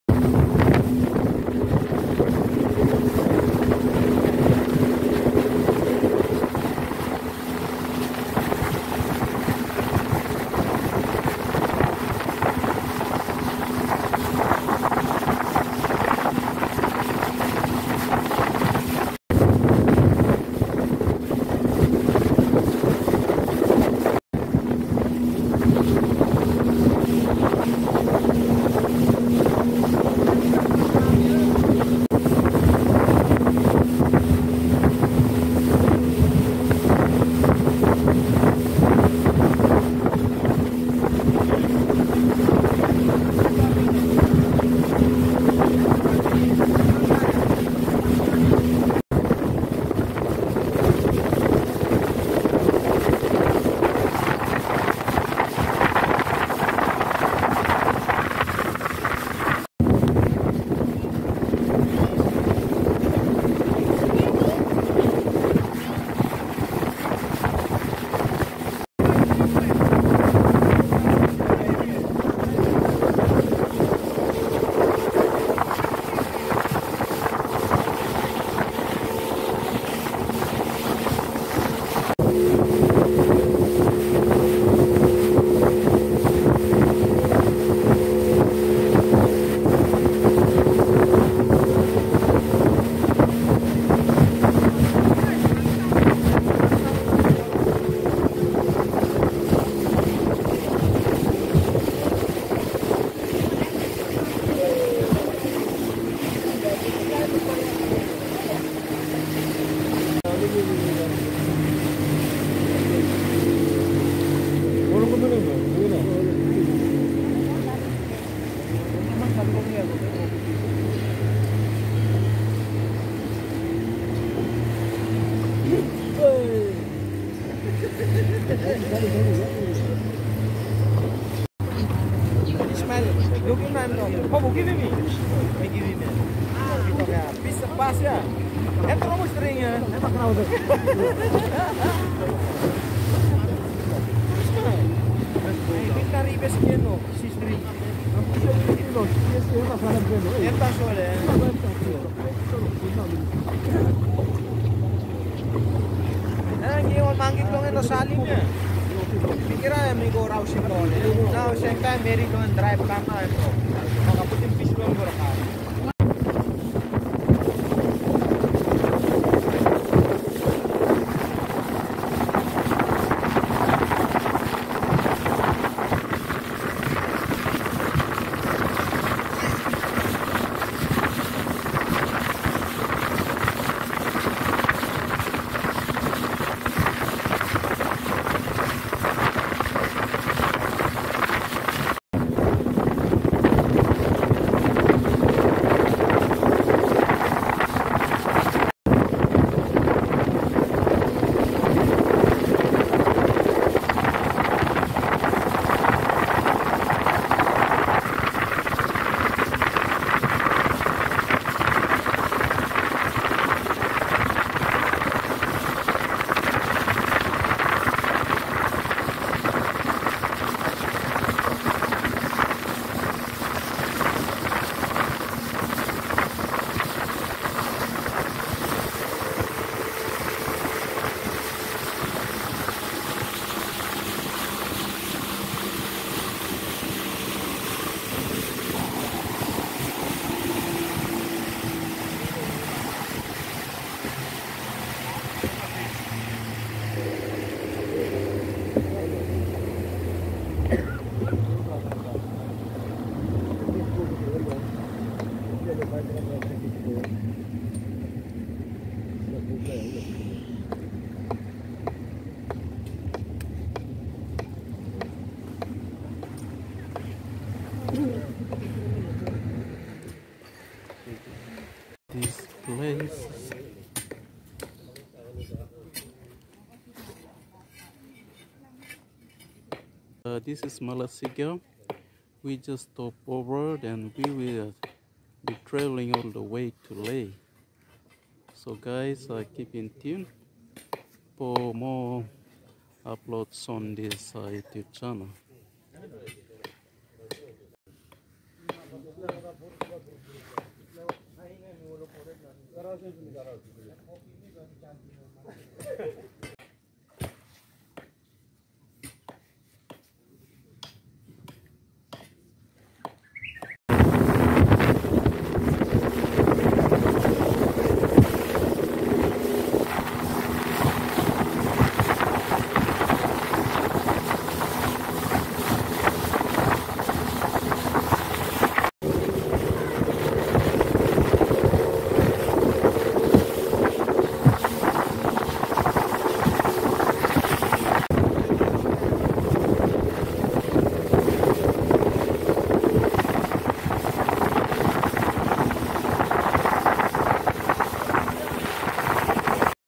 "Seafarer Wanderlust Adventure!: Papua New Guinea".
Come join @yipmabaruya1148 seafarer wanderlust adventure here on the coastal part of Finshafen. The speedboat skipper is a experience seafarer here enroute to Lae and Finschafen waters. Its always enjoyable to cruise on wanderlust adventures to remember in your life time. If anyone of you out there want to enjoy your holidays can come to Finschafen and enjoy your wanderlust sea adventures.